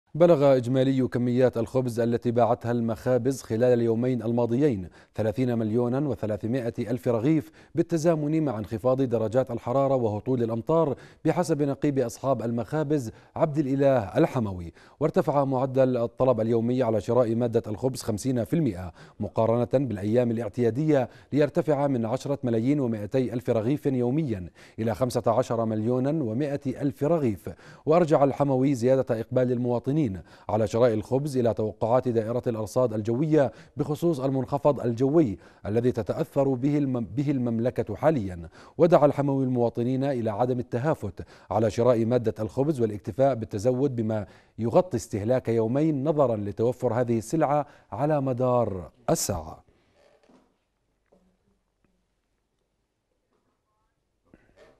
15 مليون رغيف خبز خلال يوم امس (28-1-2017)
بلغ إجمالي كميات الخبز التي باعتها المخابز خلال اليومين الماضيين 30.3 مليون رغيف، بالتزامن مع انخفاض درجات الحرارة وهطول الامطار، بحسب نقيب اصحاب المخابز عبد الإله الحموي.
وارتفع معدل الطلب اليومي على شراء مادة الخبز 50 % مقارنة بالأيام الاعتيادية ليرتفع من 10.2 مليون رغيف يوميا الى 15.1 مليون.